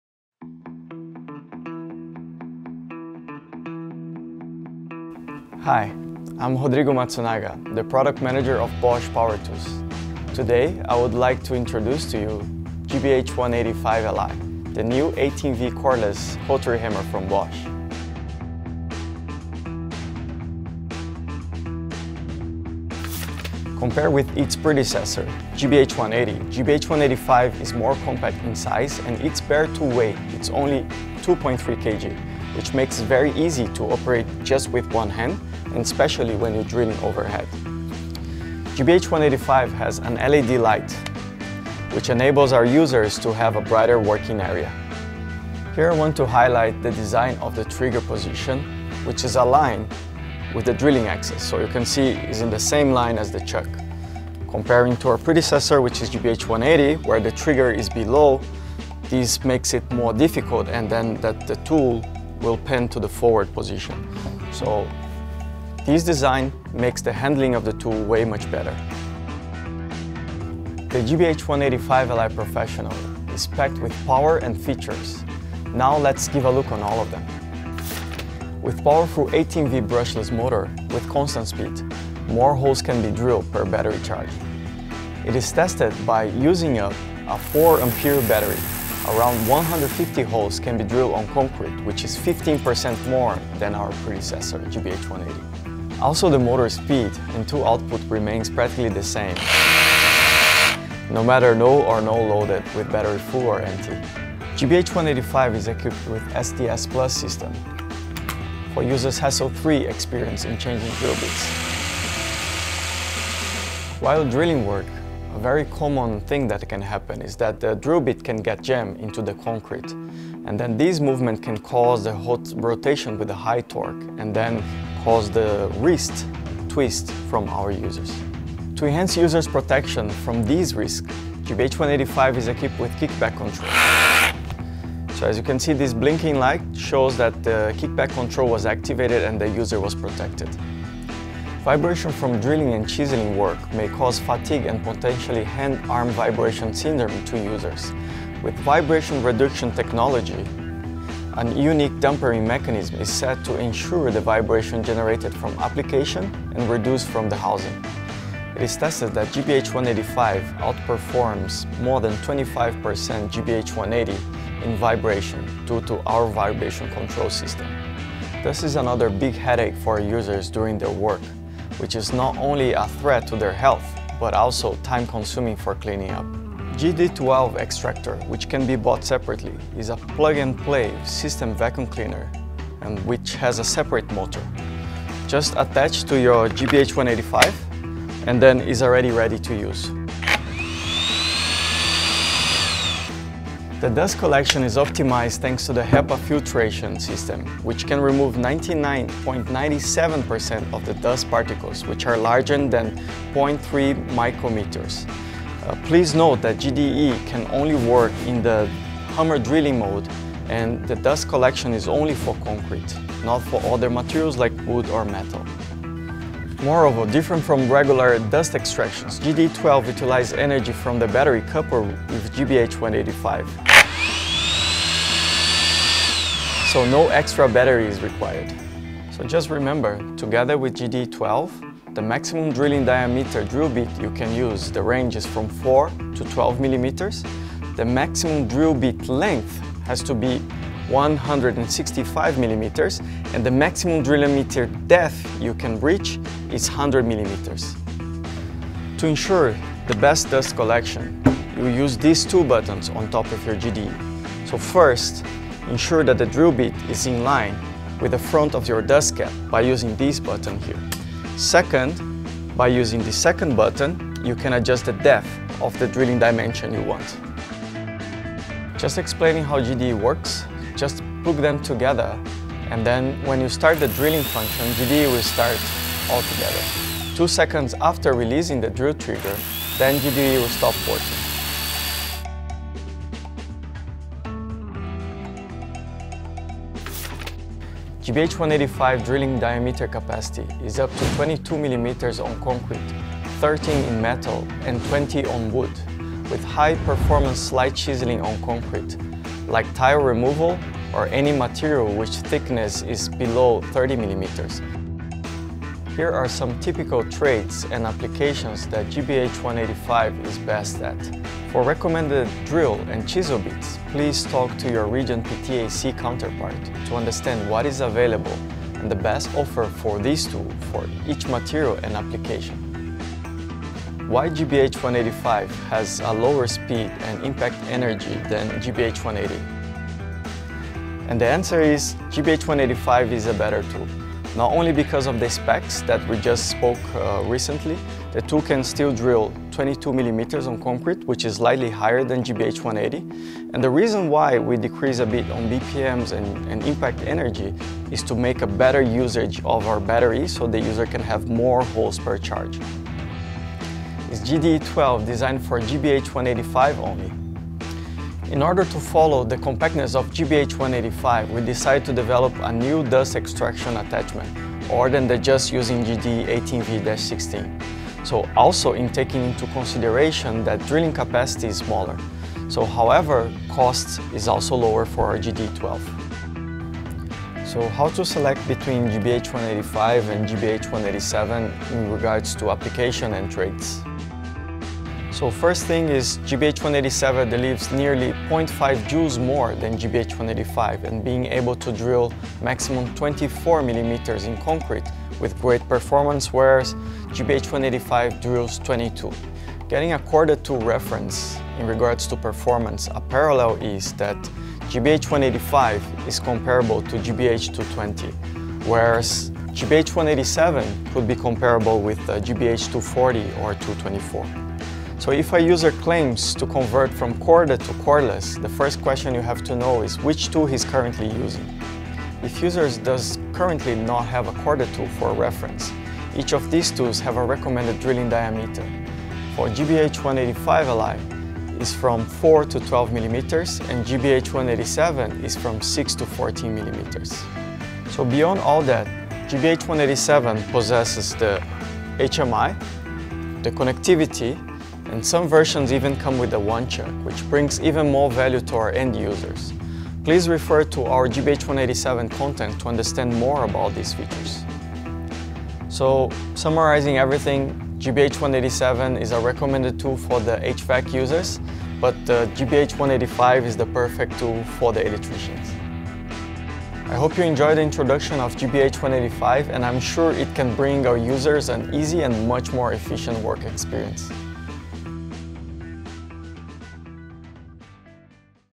REVIEW : มินิรีวิวการใช้งาน BOSCH GBH 185-LI สว่านโรตารี่ไร้สาย มอเตอร์บัสเลส ฟีเจอร์เยอะ
GBH 185-LI PROFESSIONAL
สว่านโรตารี่ไร้สายระบบ SDS PLUS
- ยกระดับการทำงานอีกขั้นด้วยประสิทธิภาพ ความปลอดภัย และความสามารถในการควบคุม
- ให้กำลังสูงอย่างต่อเนื่องด้วยมอเตอร์ความเร็วคงที่ ช่วยให้เจาะรูได้มากขึ้นต่อการชาร์จหนึ่งครั้ง
- มอบประสบการณ์ที่ดียิ่งขึ้นในงานเจาะด้วยระบบ Vibration Control ที่ออกแบบมาเพื่อดูดซับแรงสั่นสะเทือนโดยเฉพาะ
- ระบบ KickBack Control เสริมความปลอดภัยในกรณีงานติดขัด
- อุปกรณ์เสริมกักเก็บฝุ่น Auto Power GDE 12 เพื่อการทำงานที่ปราศจากฝุ่นละออง
ดูรายละเอียดเพิ่มเติมที่